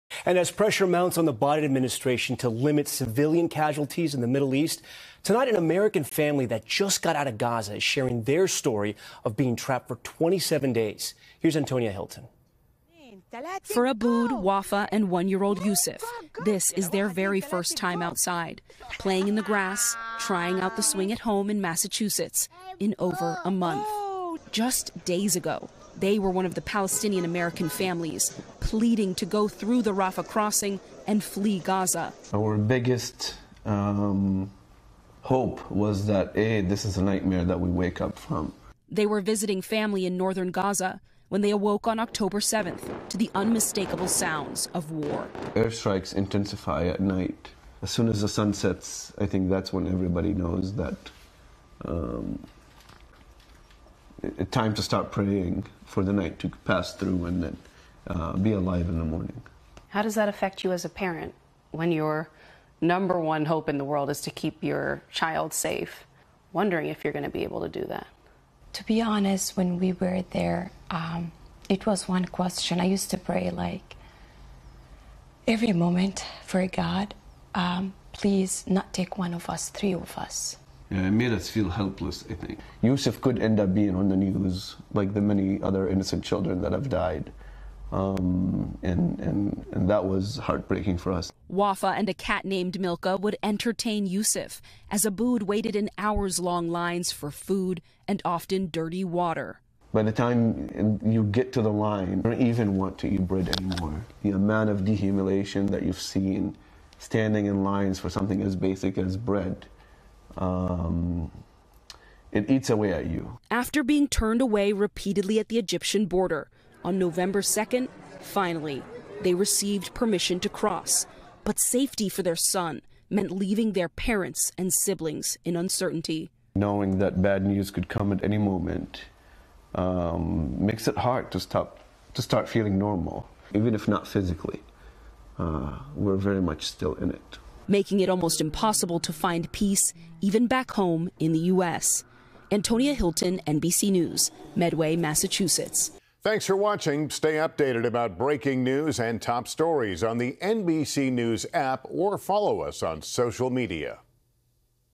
Palestinian-American family returns to U.S. after being trapped in Gaza for 27 days
A Palestinian-American family that was trapped in Gaza for 27 days is now back in the U.S. NBC News’ Antonia Hylton spoke with them about fleeing and their journey home.